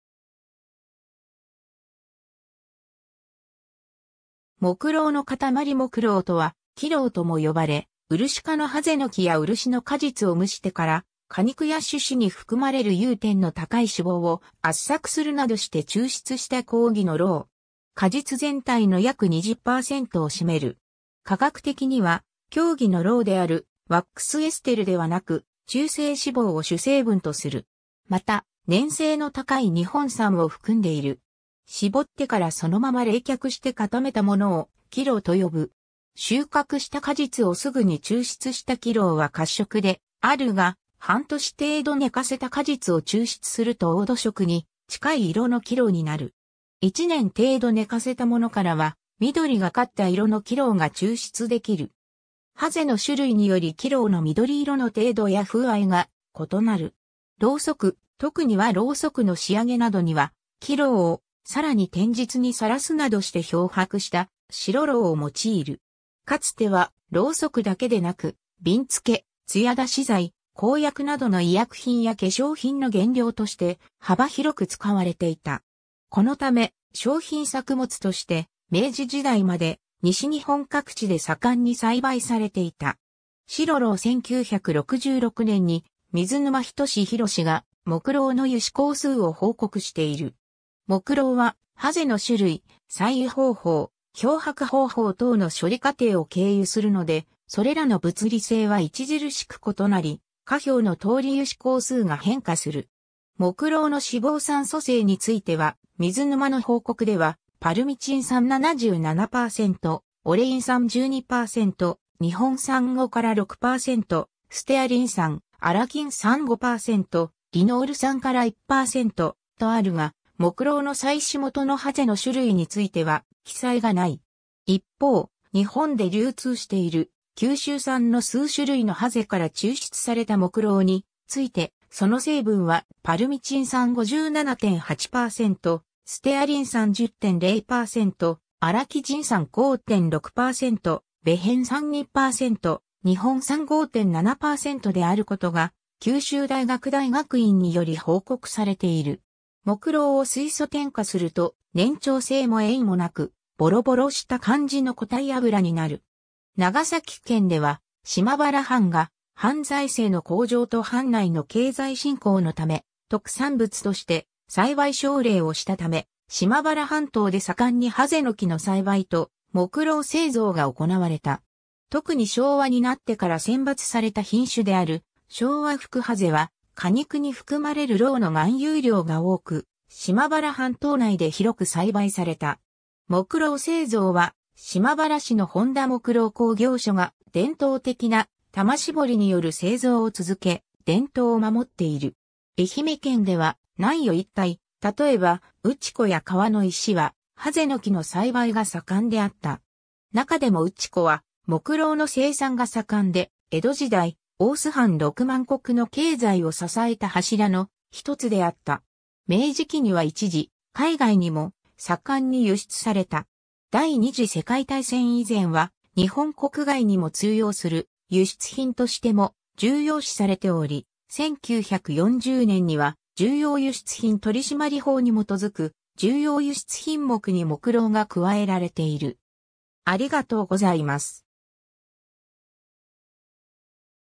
木蝋
木蝋の塊 木蝋(もくろう)（Japan wax）とは、生蝋（きろう）とも呼ばれ、ウルシ科のハゼノキ（Japanese wax tree）やウルシの果実を蒸してから、果肉や種子に含まれる融点の高い脂肪を圧搾するなどして抽出した広義の蝋。
果実全体の約20%を占める。
化学的には狭義の蝋であるワックスエステルではなく、中性脂肪（パルミチン酸、ステアリン酸、オレイン酸）を主成分とする。
また粘性の高い日本酸（Japanic acid）を含んでいる。
 搾ってからそのまま冷却して固めたものを「生蝋」（きろう）と呼ぶ。
収穫した果実をすぐに抽出した生蝋は褐色であるが、半年程度寝かせた果実を抽出すると黄土色に近い色の生蝋になる。
1年程度寝かせたものからは緑がかった色の生蝋が抽出できる。
櫨の種類により生蝋の緑色の程度や風合いが異なる。
 蝋燭、特に和ろうそく（Japanese candle）の仕上げなどには、生蝋をさらに天日にさらすなどして漂白した白蝋（はくろう）を用いる。
かつては蝋燭だけでなく、びんつけ、艶（つや）出し剤、膏薬などの医薬品や化粧品の原料として幅広く使われていた。
このため商品作物として明治時代まで西日本各地で盛んに栽培されていた。
 白蝋（木蝋を漂白したもの） 1966年に水沼仁宏が木蝋の油脂恒数を報告している。
木蝋は櫨の種類、採油方法、漂白方法等の処理過程を経由するので、それらの物理性は著しく異なり、下表のとおり油脂恒数が変化する。
 木蝋の脂肪酸組成については、水沼の報告では、パルミチン酸77%、オレイン酸12%、日本酸5～6%、ステアリン酸・アラキン酸5%、リノール酸～1%、とあるが木蝋の採取元の櫨の種類については記載がない。
一方、日本で流通している九州産の数種類の櫨から抽出された木蝋について、その成分はパルミチン酸57.8%、ステアリン酸10.0%、アラキジン酸5.6%、ベヘン酸2%、日本酸5.7%であることが九州大学大学院により報告されている。
 木蝋を水素添加すると、粘稠（ねんちゅう）性も艶もなく、ボロボロした感じの固体脂になる。
 長崎県では島原藩が藩財政の向上と藩内の経済振興のため、特産物として栽培奨励をしたため、島原半島で盛んにハゼノキの栽培と木蝋製造が行われた。
特に昭和になってから選抜された品種である「昭和福櫨」は、果肉に含まれる蝋の含有量が多く、島原半島内で広く栽培された。
木蝋製造は島原市の本多木蝋工業所が伝統的な玉絞りによる製造を続け、伝統を守っている。
 愛媛県では南予一体、例えば内子（現：内子町）や川之石（現：八幡浜市、旧・西宇和郡保内町）は、ハゼノキの栽培が盛んであった。
中でも内子は、木蝋の生産が盛んで、江戸時代、大洲藩6万石の経済を支えた柱の一つであった。
明治期には一時、海外にも盛んに輸出された。
 第二次世界大戦以前は、日本国外にも通用する輸出品としても重要視されており、1940年には重要輸出品取締法に基づく重要輸出品目に木蝋が加えられている。